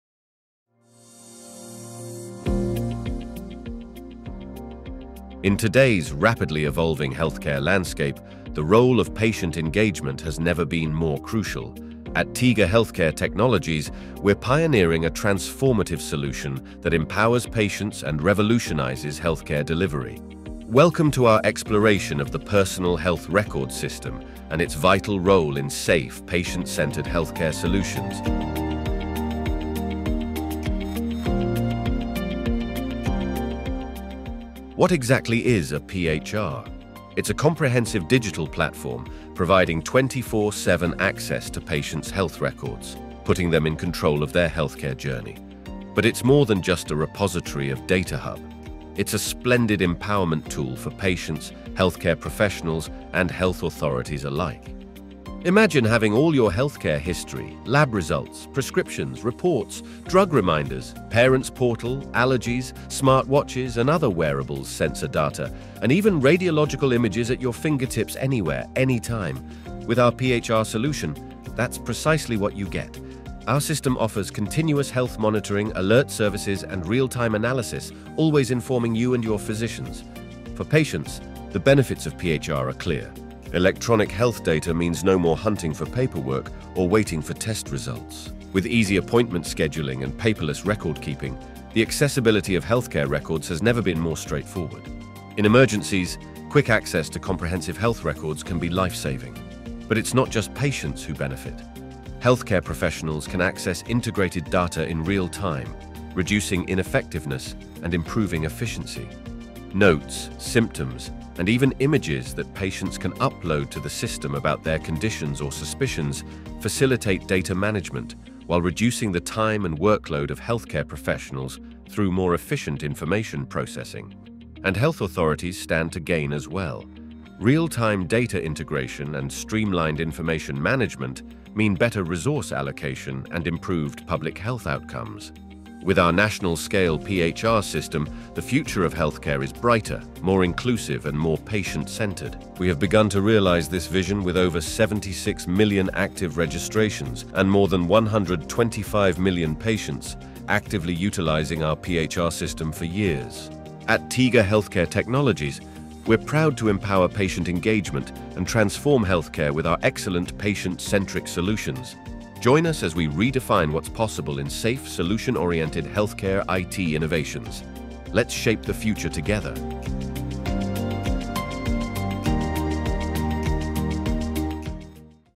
Shaping a Healthier Future: Tiga's National-Scale PHR System Transforms Healthcare Delivery
Welcome to Tiga Healthcare Technologies, where we're leading the charge in transforming healthcare delivery through innovative solutions. Join us as we delve into the world of Personal Health Record (PHR) Systems and their indispensable role in fostering safe and patient-centered healthcare.

In today's fast-paced healthcare landscape, patient engagement is more critical than ever. Our PHR system provides patients with 24/7 access to their health records, empowering them to take control of their healthcare journey. But it's more than just a data hub – it's a powerful tool for patients, healthcare professionals, and health authorities alike.

Imagine having all your health information, from lab results to medication reminders, right at your fingertips. With our PHR solution, that's exactly what you get. Plus, with continuous health monitoring and real-time analysis, you and your physicians are always informed.

The benefits of PHR extend beyond patients. Healthcare professionals can access integrated data in real-time, reducing inefficiencies and improving effectiveness. And for health authorities, streamlined information management leads to better resource allocation and public health outcomes.

Join us as we shape the future of healthcare with our national-scale PHR system, already trusted by millions of patients. At Tiga Healthcare Technologies, we're dedicated to empowering patient engagement and revolutionizing healthcare delivery. Together, let's redefine what's possible in safe and patient-centered healthcare IT innovations. Join us today!